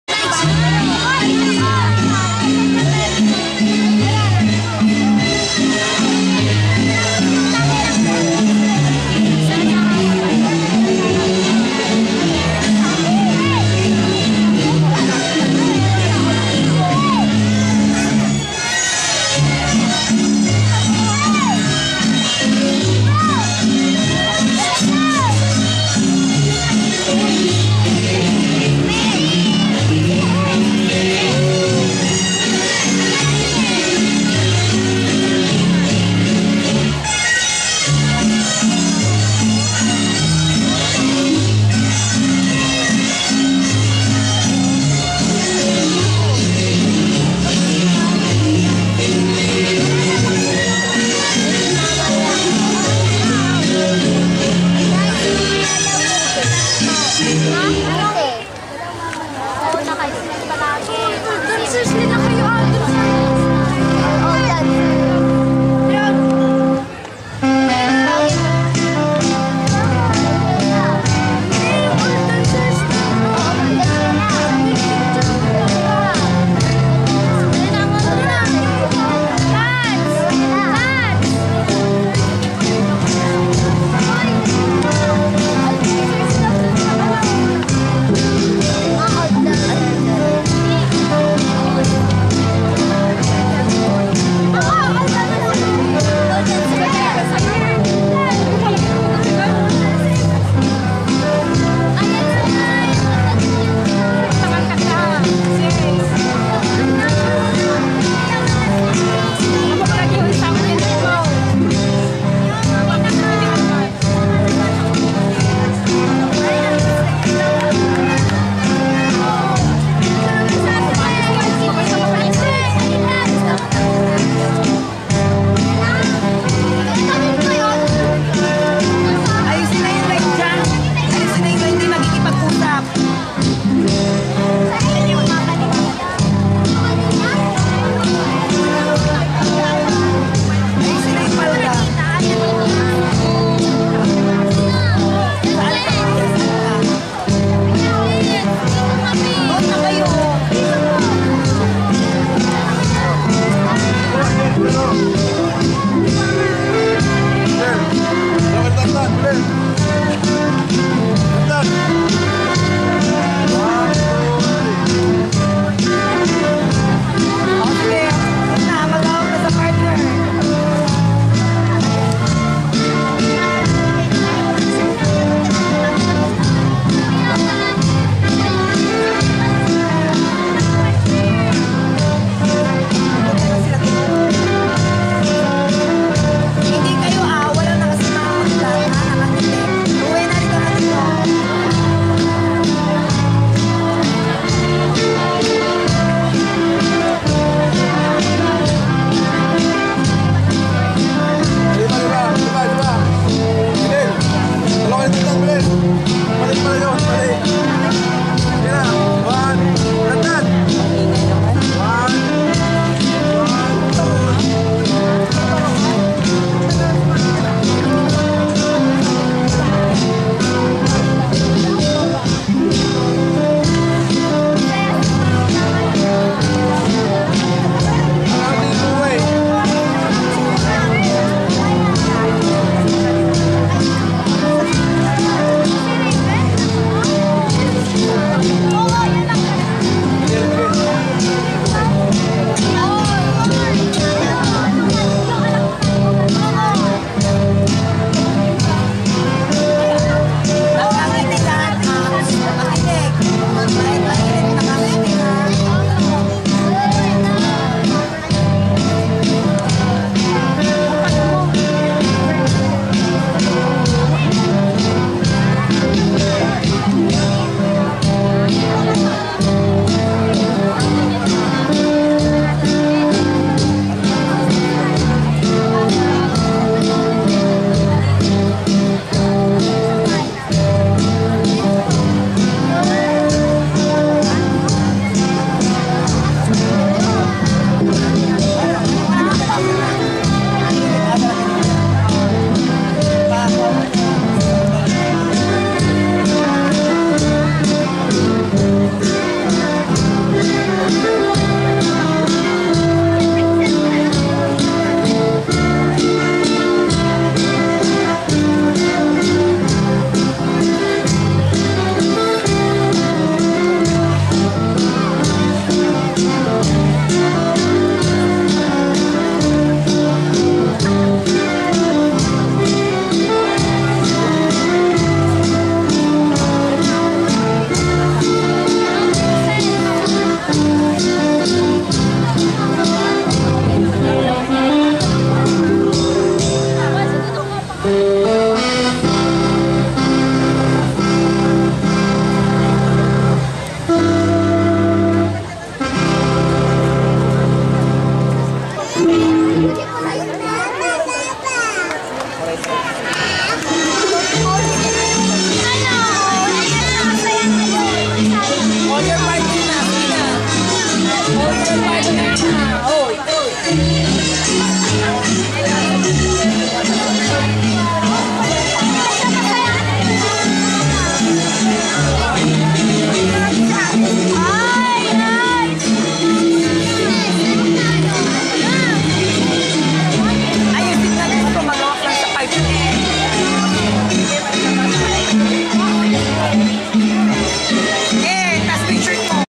Juan C Laya Central School's Folk Dance entry
The Juan C. Laya learners' Folk Dance performance at San Manuel town fiesta, they finished as the champion